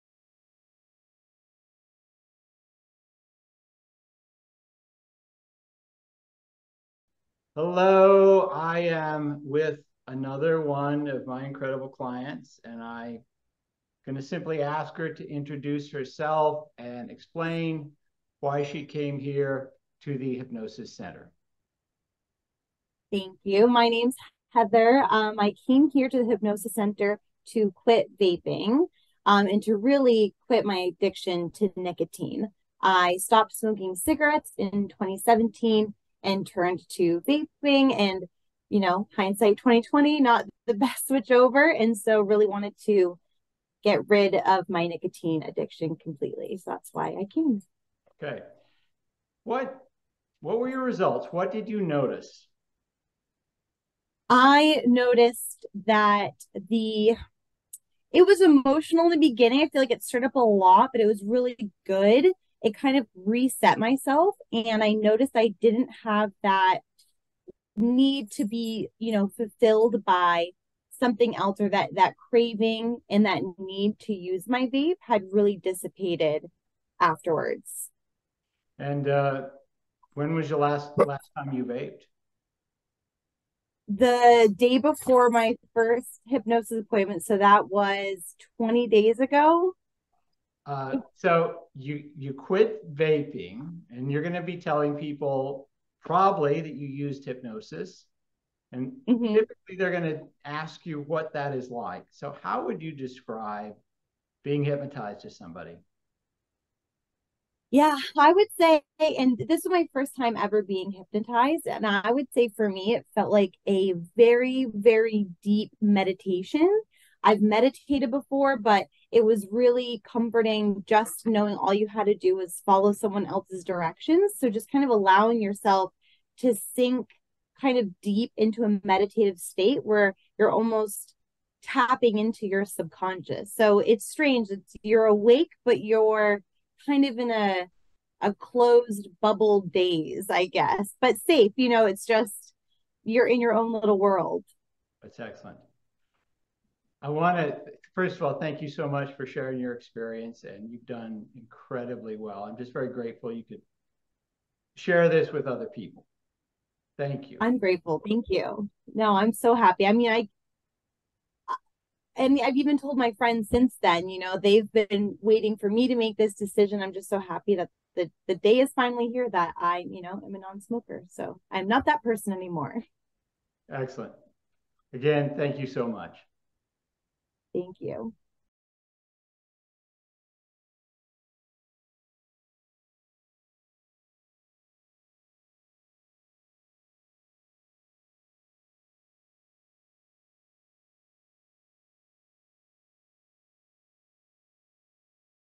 Heather Quits Vaping Using Hypnosis
Hypnosis can be done successfully in person or through teleconferencing services like Zoom and Skype. Ask today about how it works.